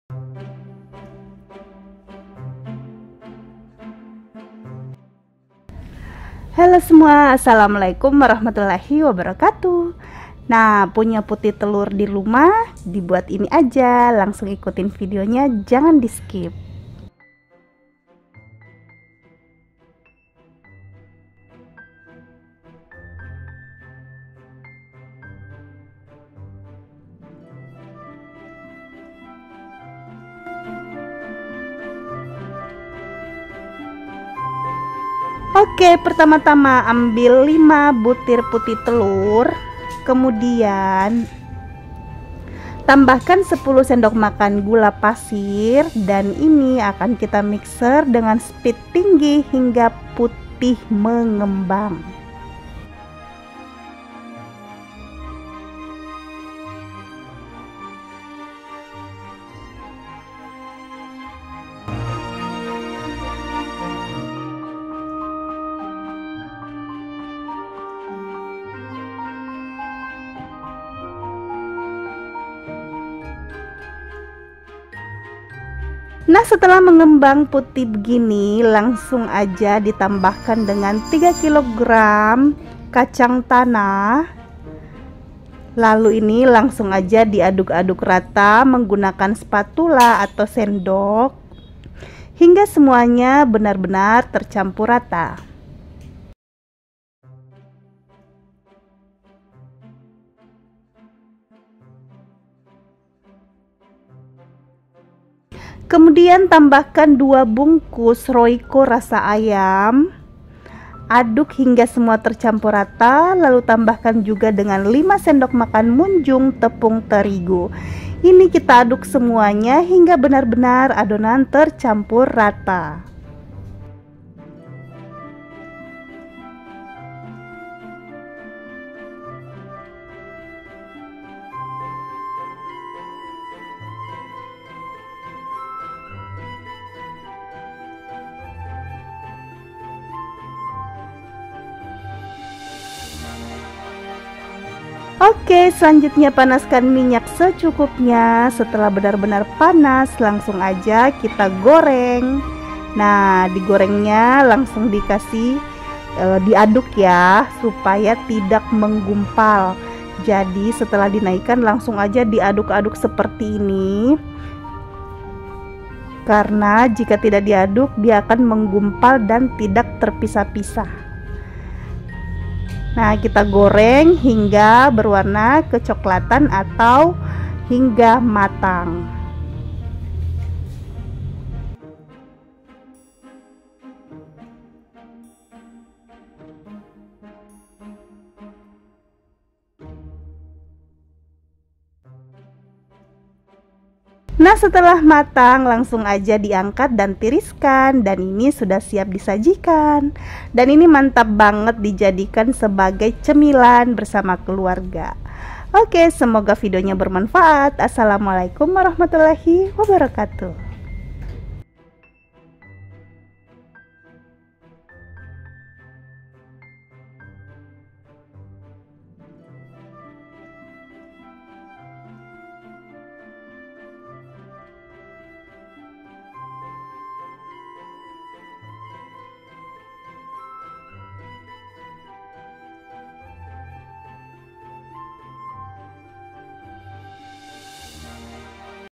PUNYA PUTIH TELUR YUKS DIBUAT INI SAJA CEMILAN UNTUK KELUARGA TERCINTA
Bahan-bahan
5 btr putih telur
0 sdm gula pasir
5 sdm munjung terigu
3 kg kacang tanah
2 bks royco ayam